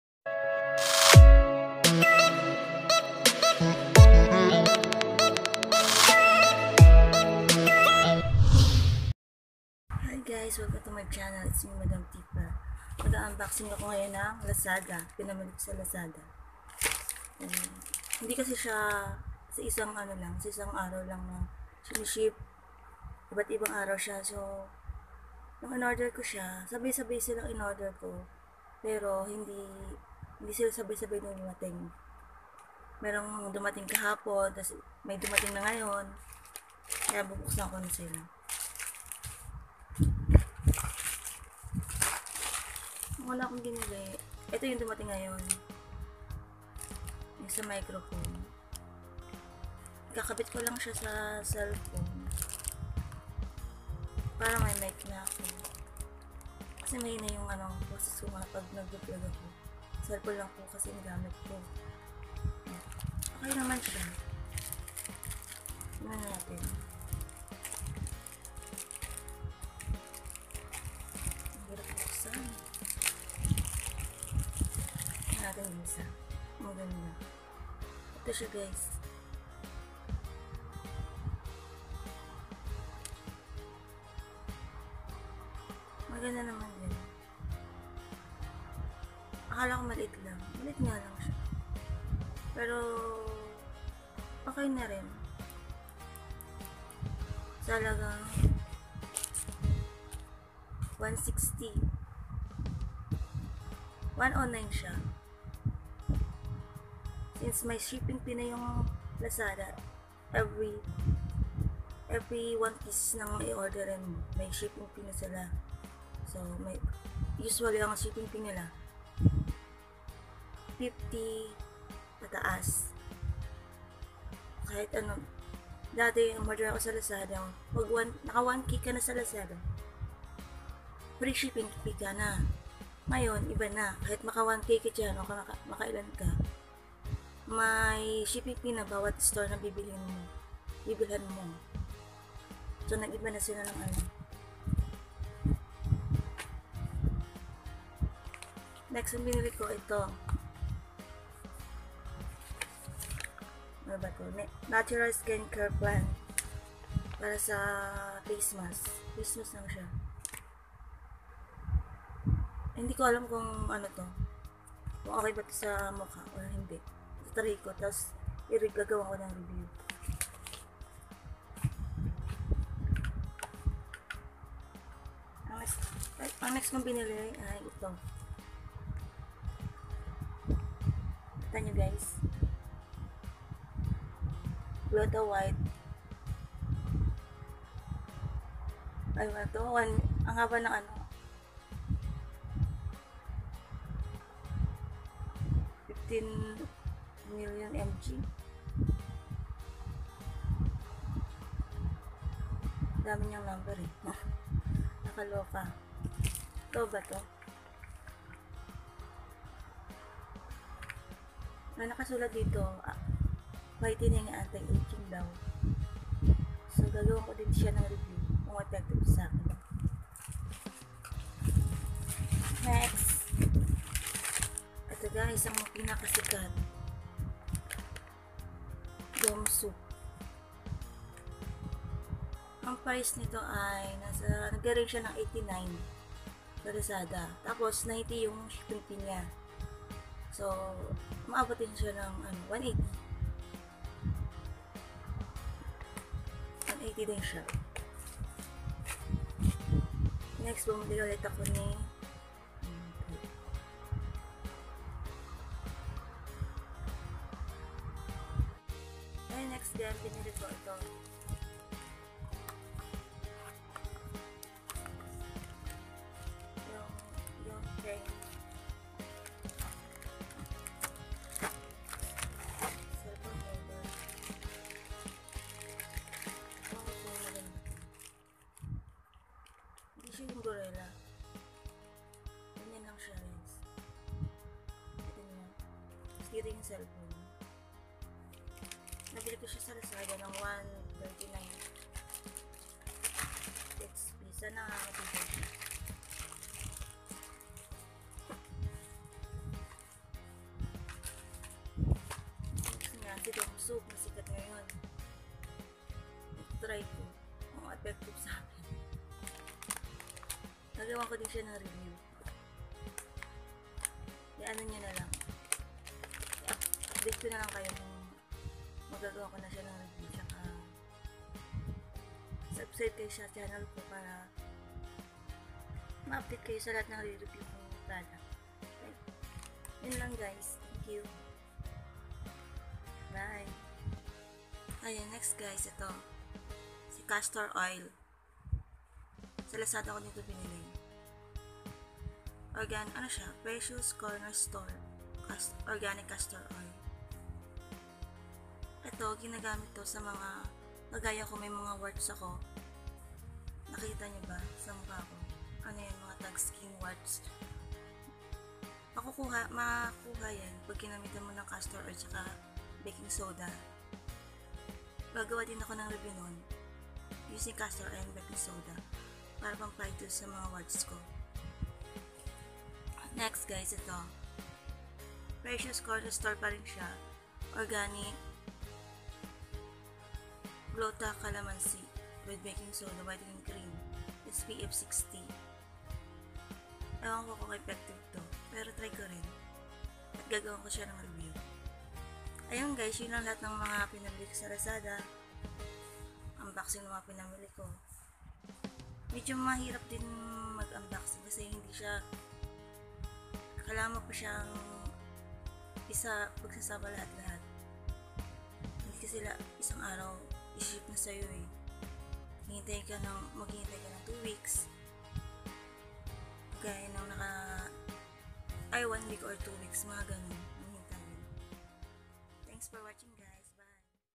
LAZADA HAUL UNBOXING
UNBOXING my lazada haul tayo ngayon guys lahat ng pinamili ko sa lazada ipapakita ko sa inyo nasa ibaba yung mga price .

Mini microphone -109php +50php shipping fee
Yunfeng mini tripod - 139php + 50php shipping  fee
Supreme Gluta White 1500000mg - 349php+ 50phpsf
Castor oil 50ml- 75php +50sf
Gluta kalamansi cream SPF 60- 75php +50 sf
Natural skincare plant face mask - 49php 50 sf
Nagbabago po ang price ng bawat isa.

Frince Lyn Romero
Blk 11 lot 30 phase II
carmona estate bgry Lantic
carmona cavite 4116